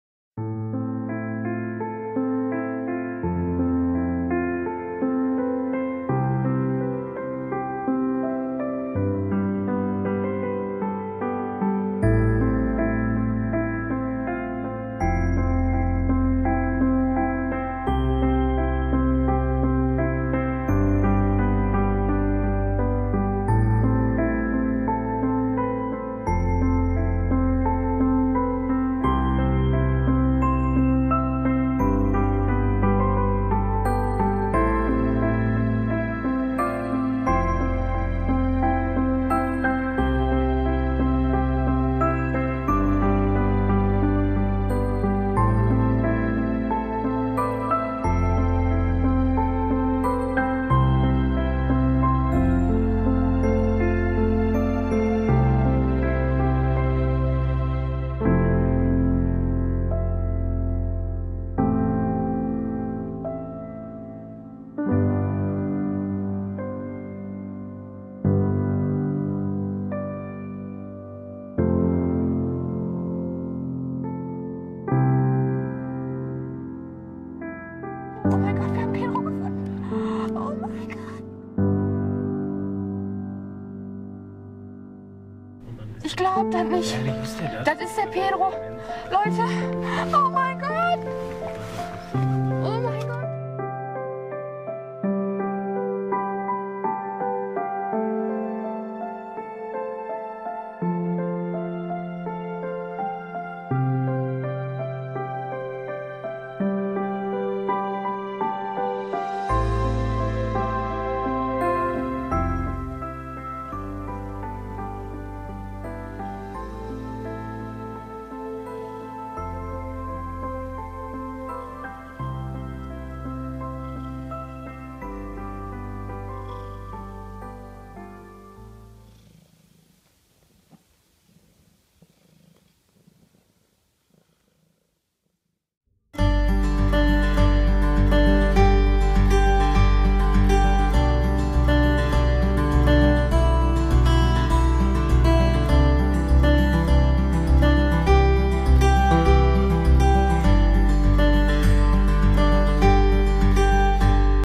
Endlich Zuhause! Kater wird gefunden!
♥Kommentar:

Was haben wir Tränen der Freude geweint!

UNFASSBAR!
Nach 407 Tagen haben wir ihn gefunden! Von unseren Bekannten wussten wir, dass er angeblich dort in der Nähe den Kater gesehen hatte, aber wir fanden ihn dort einfach nicht. Mehrere Monate intensive Suche, der ganze Ort bei uns suchte anch ihm, Fallen wurden aufgestellt aber nein er war nie drin. Tierheime wussten Bescheid, alle Tierschutzvereine in der Umgebung, die Tierärzte udn so viele haben uns geholfen ihn zu suchen aber einfach NICHTS...

Da saß er dann zusammengekauert neben den Bahngleisen und als man seinen Namen rief, antwortete er direkt mit einem Mauzen.

Er hat die ganze Autofahrt über laut geschnurrt und mich mit seinen Pfoten massiert :3

Pedro ist ein Britisch-Europäisch Kurzhaarmix und hat längeres Fell, weshalb er so muskolös wirkt. Aber nein, er war nur noch Haut und Knochen, jeden Rückenwirbel konnte man spüren und seine Kopfform war gar nicht mehr rund. Er hatte Flöhe und saß voller Würmer, die seinen Bauch aufgedunsen hatten. Jetzt ist er versorgt und weitere Infos über ihn werdet ihr nach Weihnachten auf meinem Kanal finden.